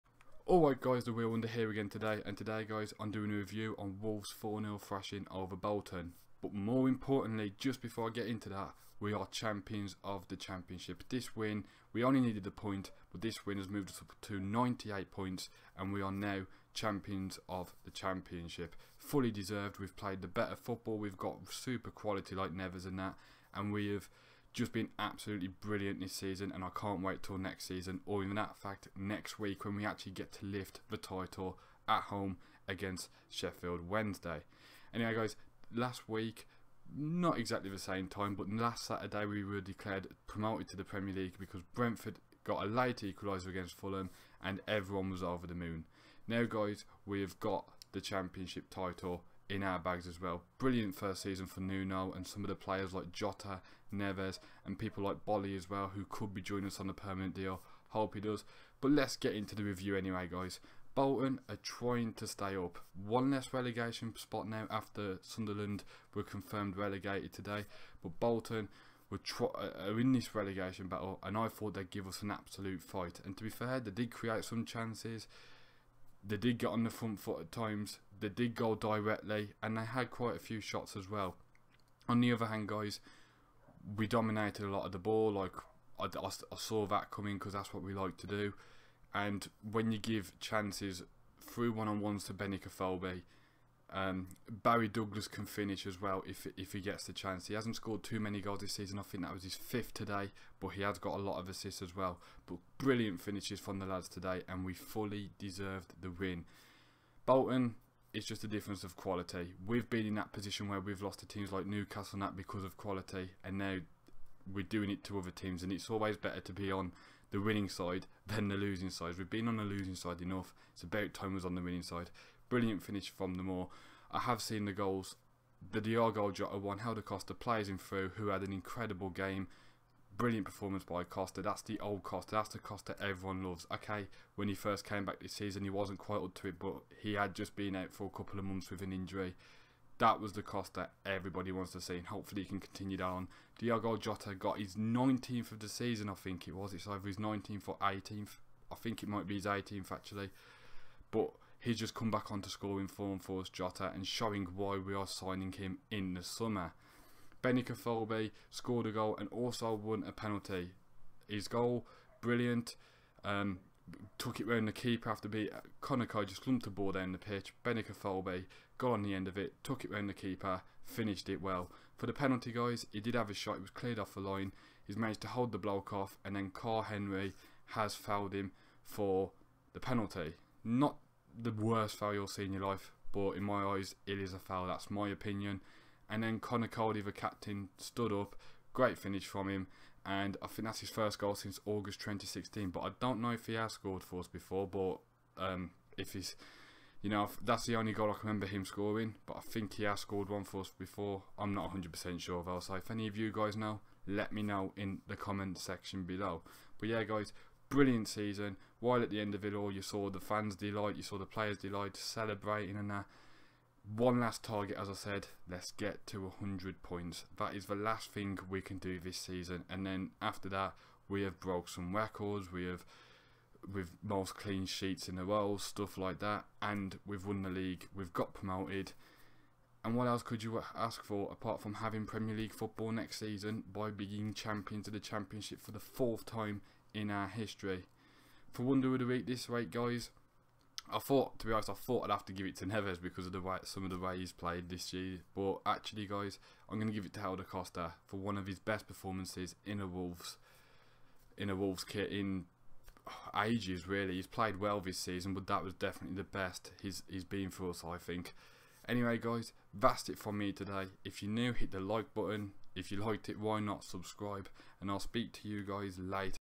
WE ARE THE CHAMPIONS - Bolton 0-4 Wolves Review!!!!
Wolves secure the championship title with a 4-0 away win at Bolton!